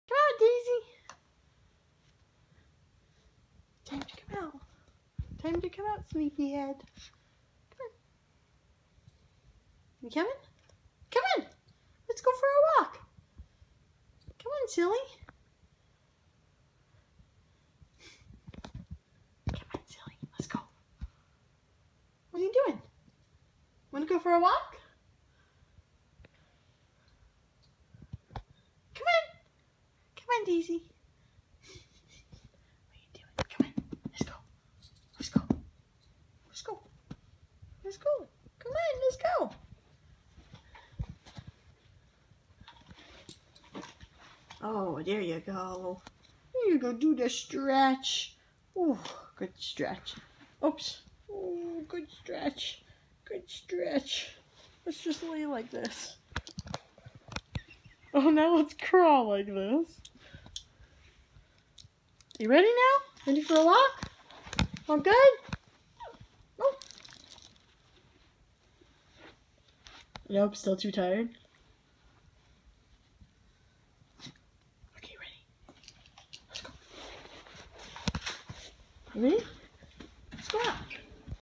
Beagle puppy struggles to wake up!
Daisy was too tired today. Watch for her funny stretch at the end!